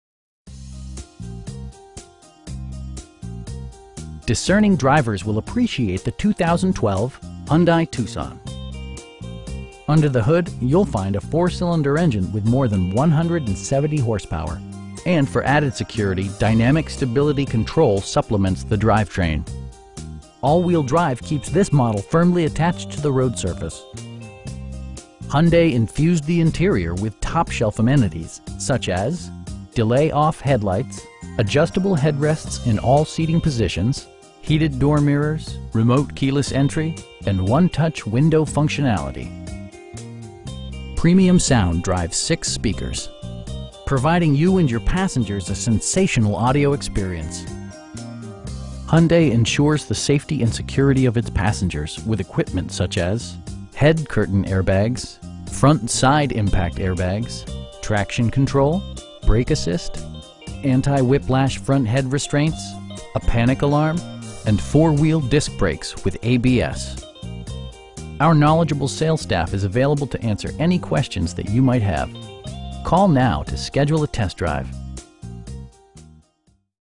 2012 Hyundai Tucson GLS in Fayetteville, AR 72703
Superior Kia
3195 N. College Ave.

Discerning drivers will appreciate the 2012 Hyundai Tucson. Under the hood you'll find a 4 cylinder engine with more than 170 horsepower, and for added security, dynamic Stability Control supplements the drivetrain. All wheel drive keeps this model firmly attached to the road surface. Hyundai infused the interior with top shelf amenities, such as: delay-off headlights, adjustable headrests in all seating positions, heated door mirrors, power windows, remote keyless entry, an overhead console, and 1-touch window functionality. Premium sound drives 6 speakers, providing you and your passengers a sensational audio experience. Hyundai ensures the safety and security of its passengers with equipment such as: head curtain airbags, front SIDE impact airbags, traction control, brake assist, anti-whiplash front head restraints, a panic alarm, and 4 wheel disc brakes with ABS. Our knowledgeable sales staff is available to answer any questions that you might have. They'll work with you to find the right vehicle at a price you can afford.